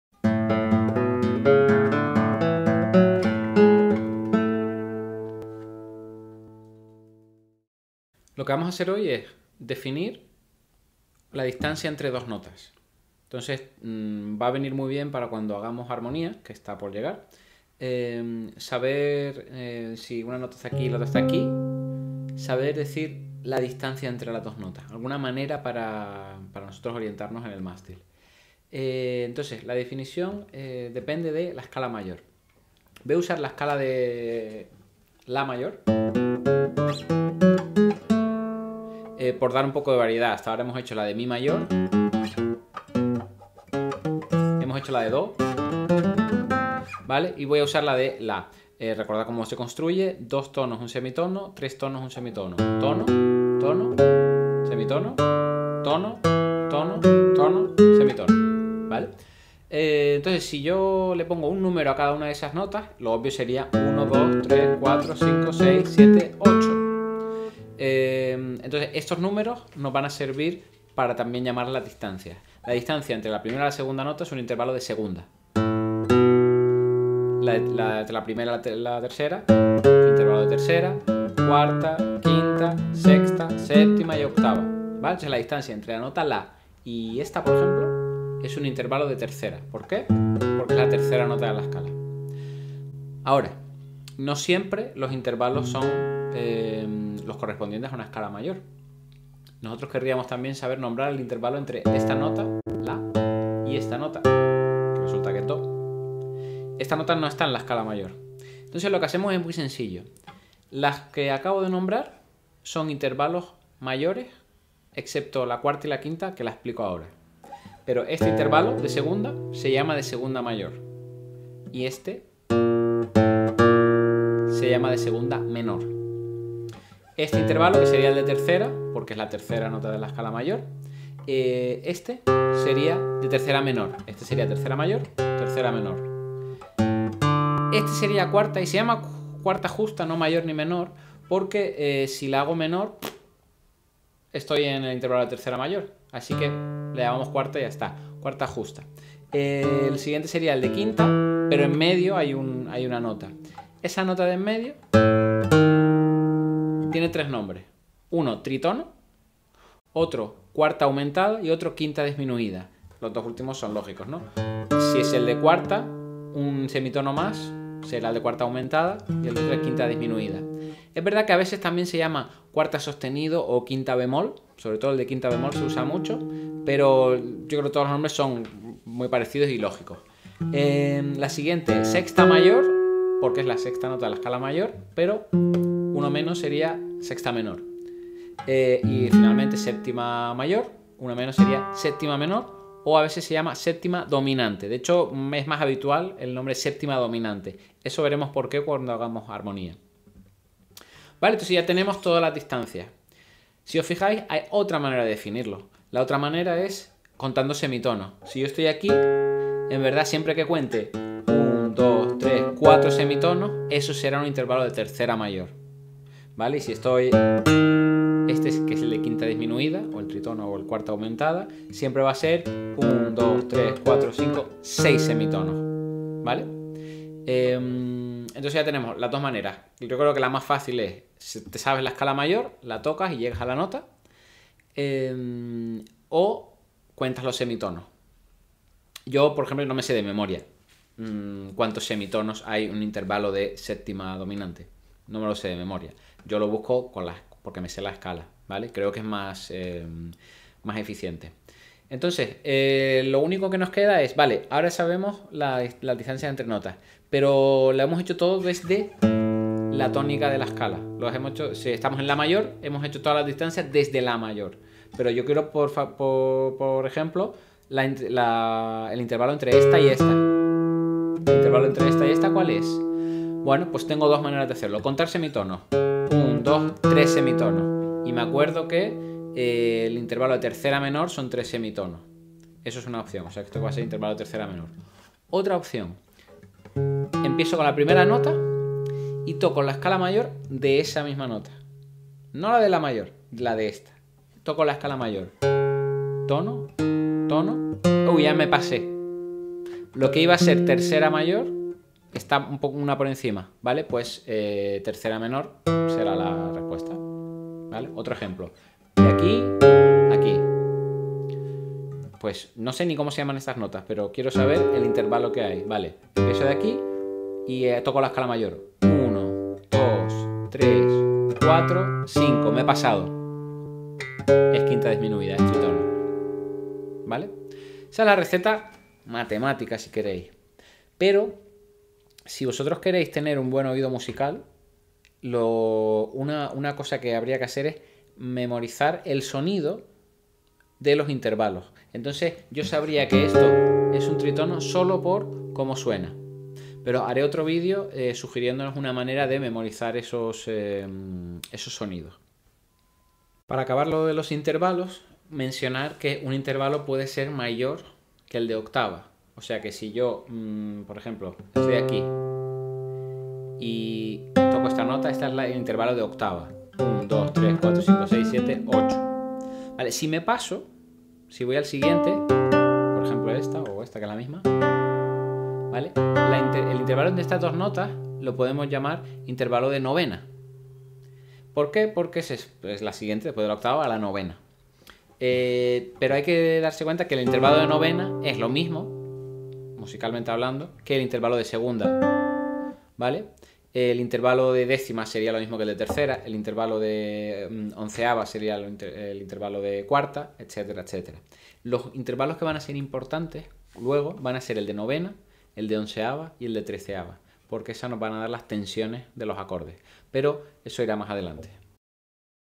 Intervalos entre notas
Aquí explico el concepto de intervalo como la distancia entre dos notas. Esto nos será útil para hablar de armonía.